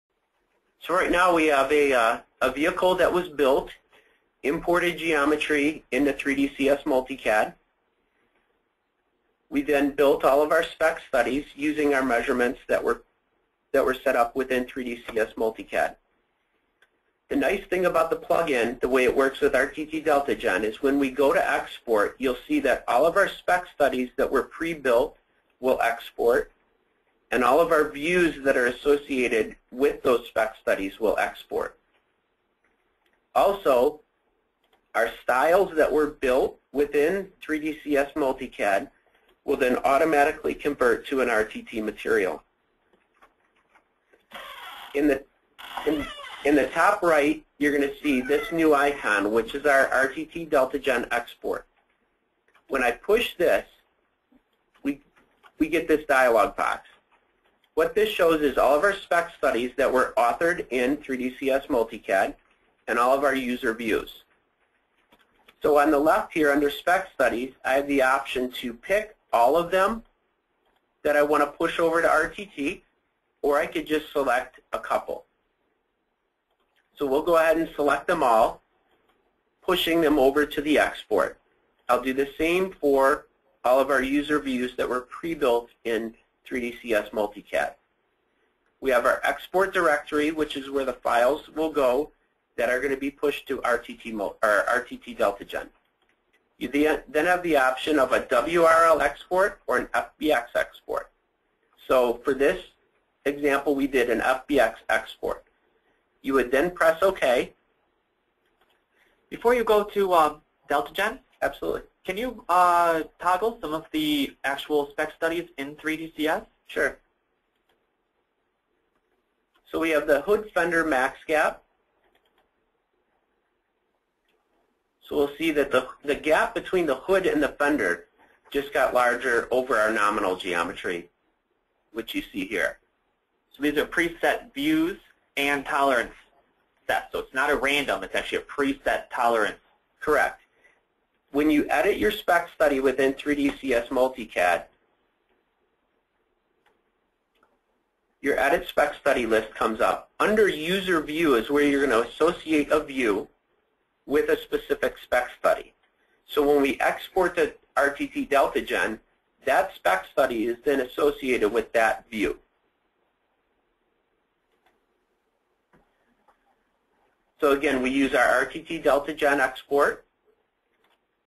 2. Spec Study Export to Deltagen
Demonstrating Spec Studies in 3DCS Multi-CAD, these are then exported to 3DEXCITE's Deltagen using the new 3DCS Add-on and the push of a button.